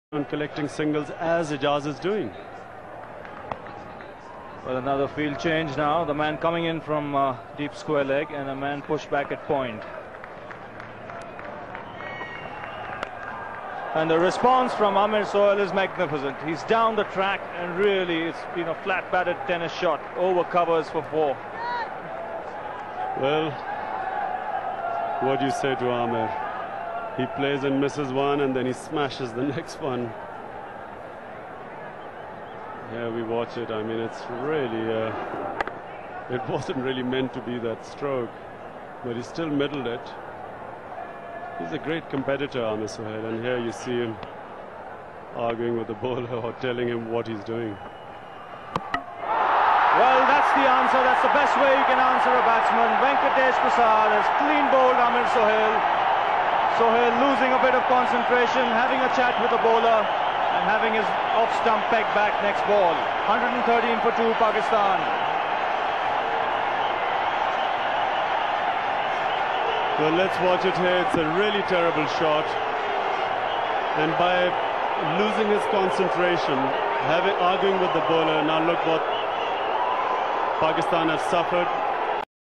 Ind vs Pak 1996 World Cup QF Amir Sohail vs Venkatesh Prasad off stump moment
The match is memorable, Jadeja literally broke the roof at the Bangalore stadium, then Aamir Sohail’s bravado moment, and at the very next ball, the way that off stump flew off, it is the closest I have come to a religious experience.